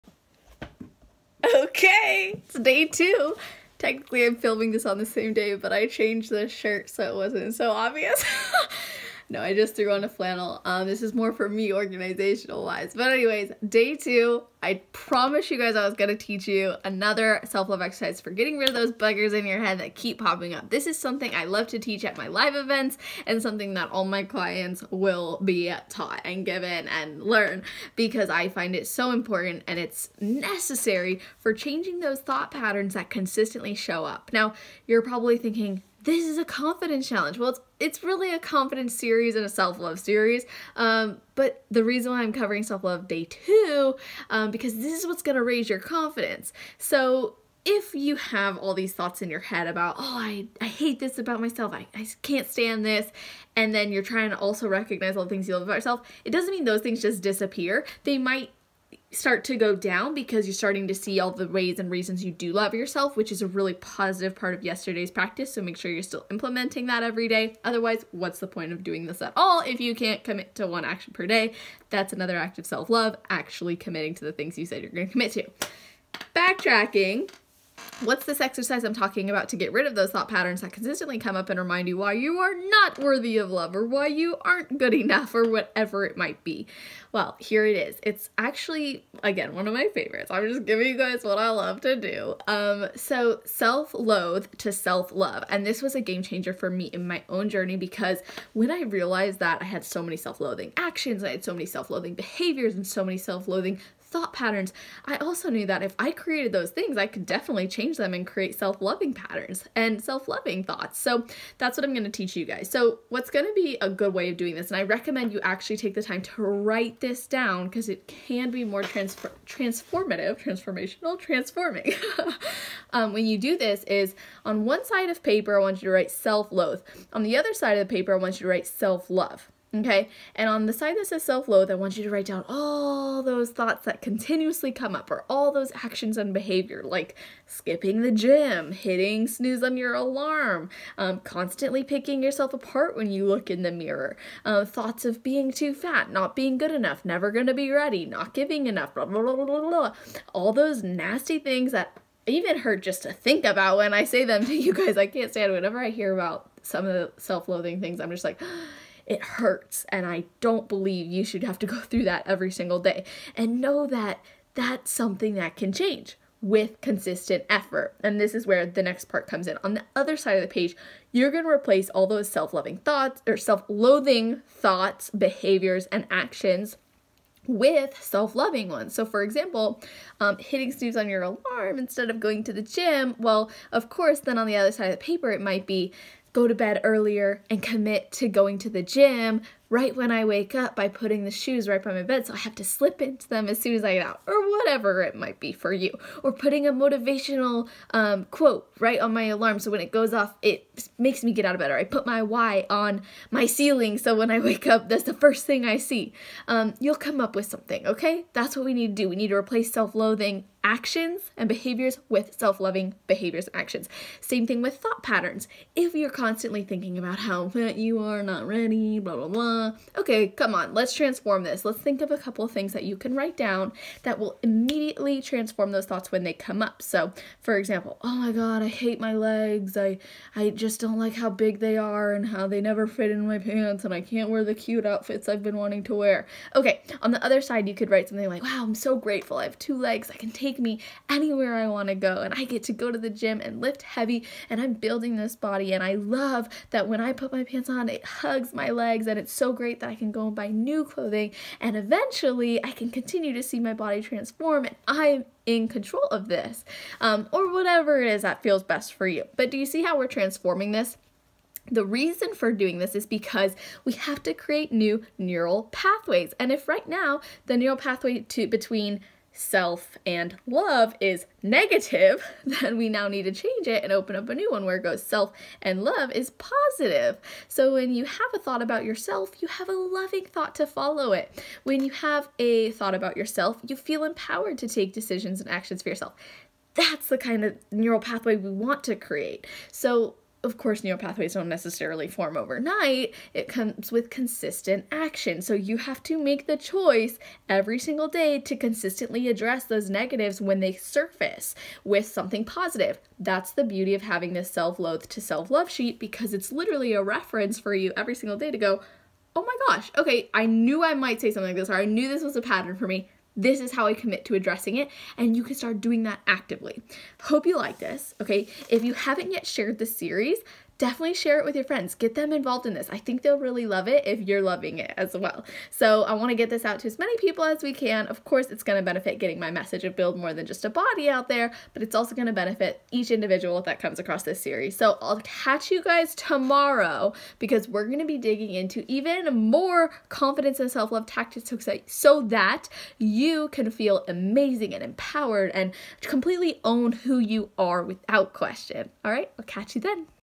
CONFIDENCE & SELF-LOVE SERIES Episode 2: Transforming Self-Loathe to Self-Love
Day 2 of the series is all about transforming loathe to love with one of my favorite exercises!
Let me know when you’ve completed this!